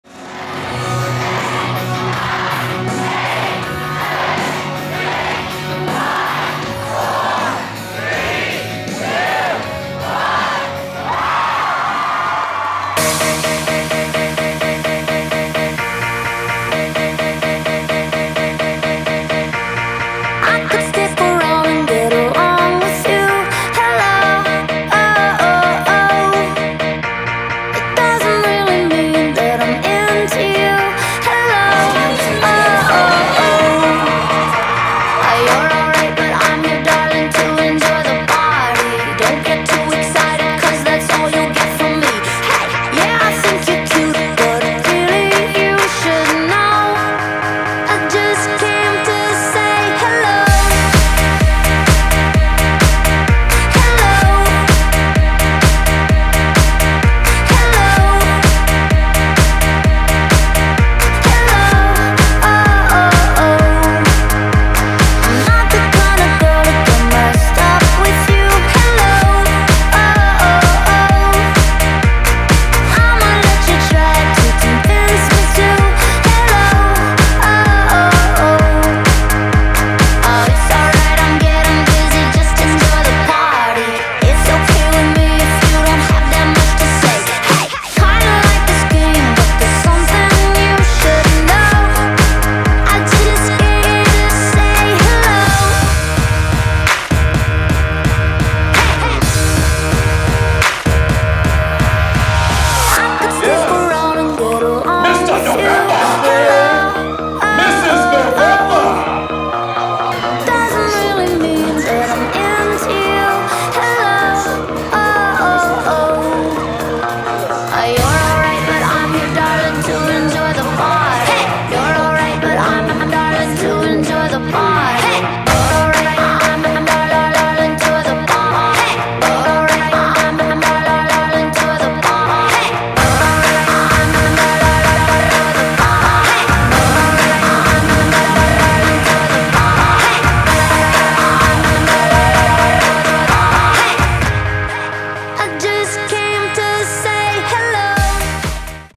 Came to say hello: Bellerose Bike-a-Thon 8 Closing Video
Way to go Bellerose!  You raised $130,000 for the Alberta Cancer Foundation as well as Kids with Cancer Organization!  After 48 hours, close to 600 of your classmates accomplished something very few students have ever done!

Be proud... Be Bellerose!